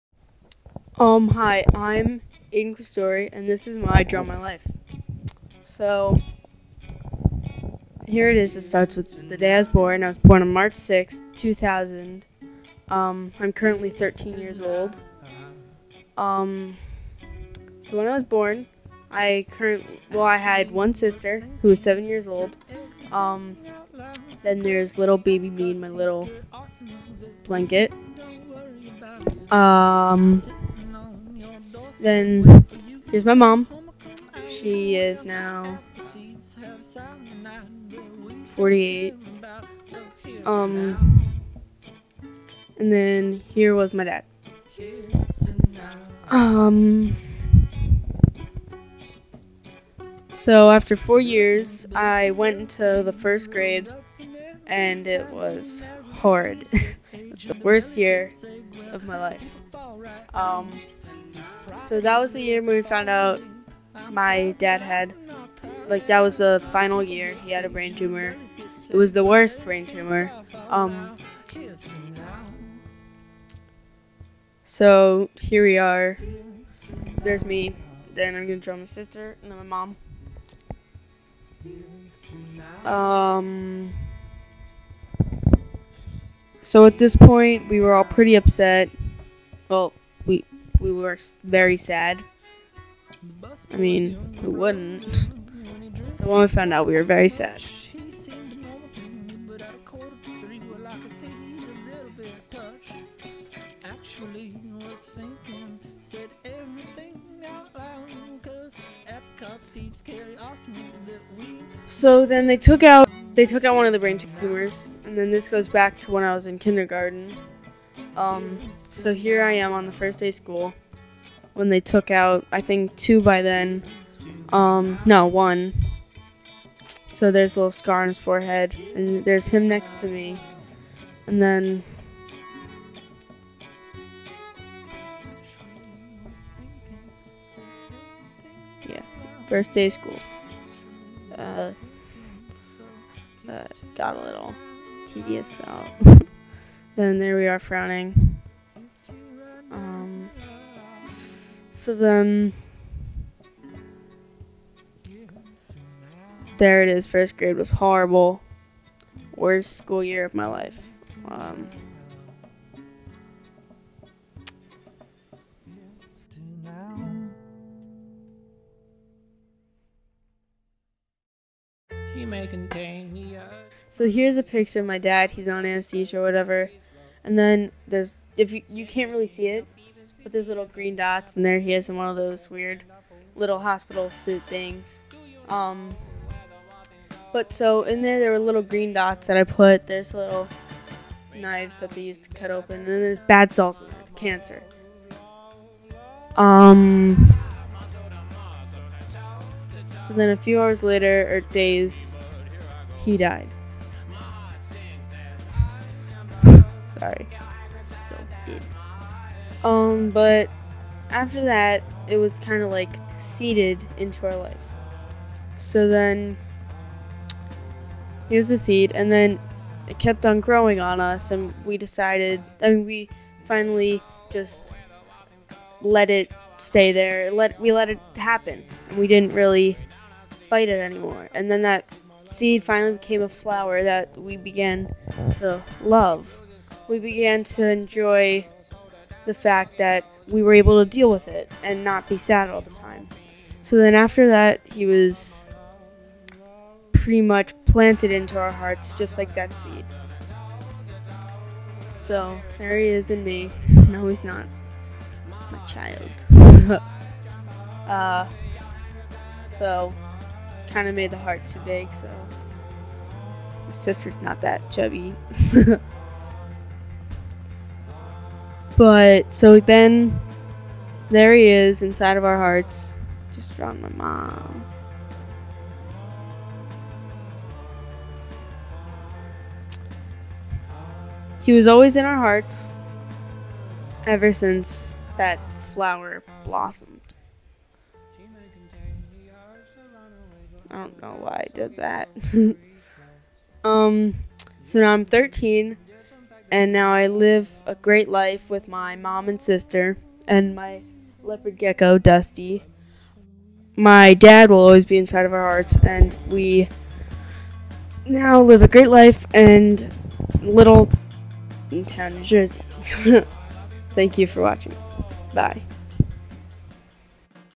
Aidan's Draw My Life
Aidan Questore was 5 years old when his Dad was diagnosed with a GBM (Glioblastoma Multiforme), eventually passing away from the brain tumor. In this video he produced at Camp Jinka, Aidan tells the story from his perspective from diagnosis, from the illness and surgery, to death, and finally to the beautiful way he and his family found peace.